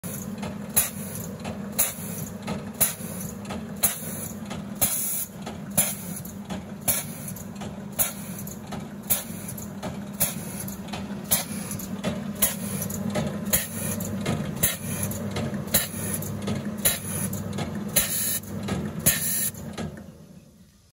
Heto machine running with Ellepot membrane
Ellepot bio-membrane is very suitable for Heto machines